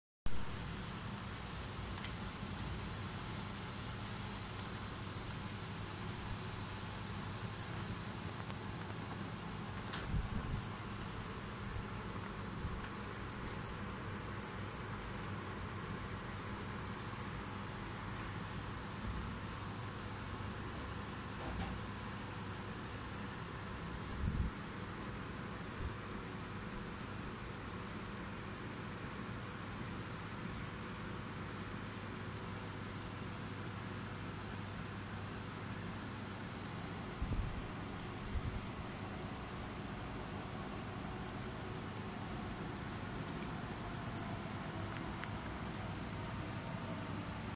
Tabby Cat licking his fish toy
Tabby cat is playing and licking his fish toy